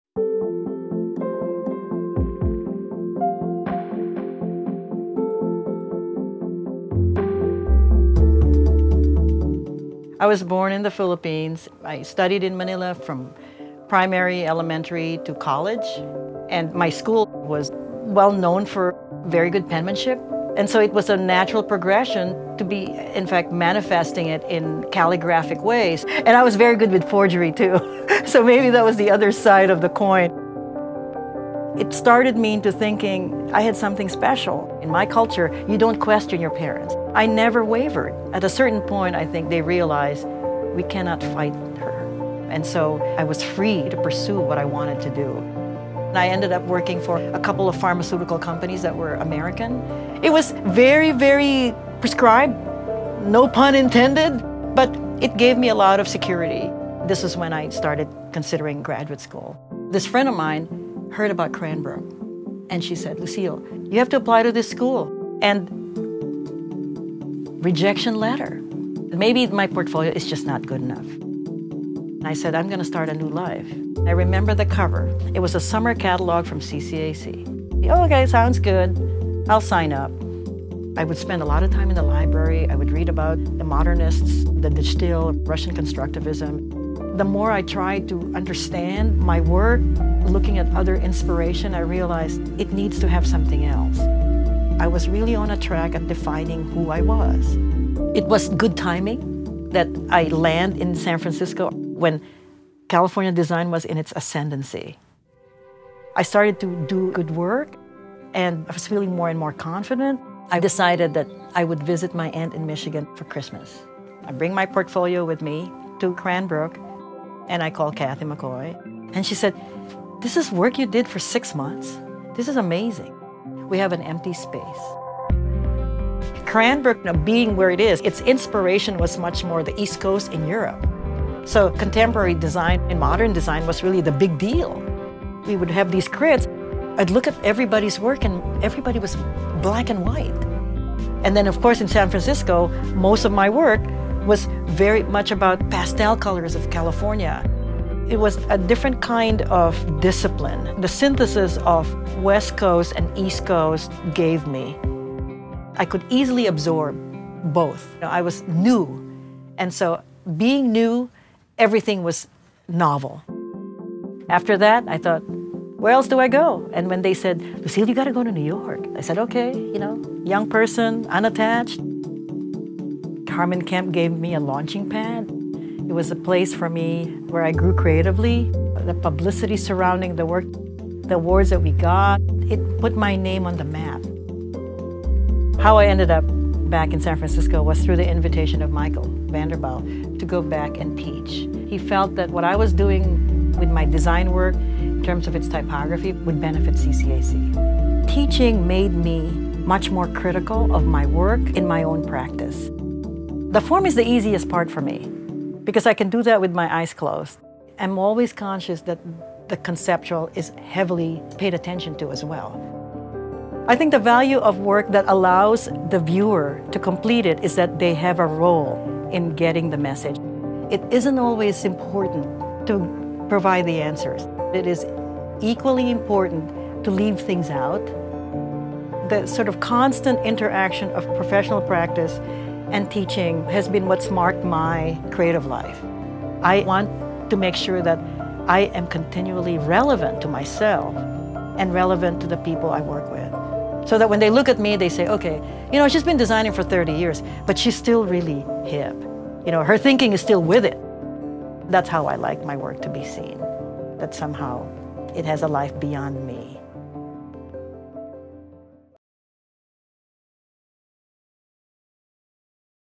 Lucille Tenazas: 2013 AIGA Medalist
Design educator and practitioner Lucille Tenazas is recognized for her prominent role in translating postmodern ideas into critical design practice; her exploration of the relationship between type, photography and language; and the development and leadership of highly-respected design education programs—always with exquisite execution.

This video by Dress Code premiered in 2013 at "The AIGA Centenial Gala" in New York City, where Tenazas was presented with the award.